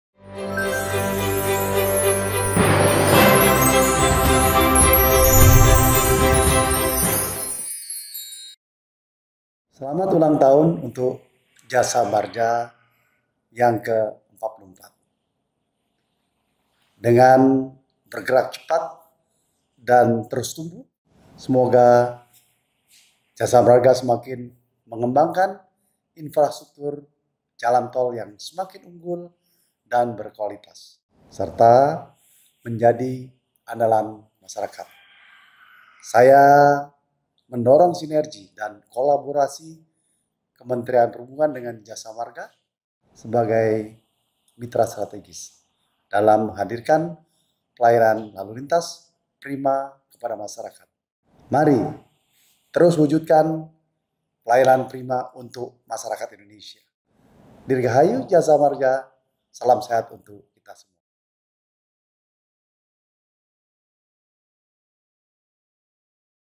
Ucapan Menteri Perhubungan Republik Indonesia, Bapak Budi Karya Sumadi untuk Jasa Marga
Terima kasih kami sampaikan untuk Menteri Perhubungan Republik Indonesia, Bapak Budi Karya Sumadi, atas ucapan dan doanya untuk Ulang Tahun ke-44 PT Jasa Marga (Persero) Tbk.

“Dengan bergerak cepat dan terus tumbuh, semoga Jasa Marga semakin mengembangkan infrastruktur jalan tol yang semakin unggul dan berkualitas serta menjadi andalan masyarakat,” — Budi Karya Sumadi.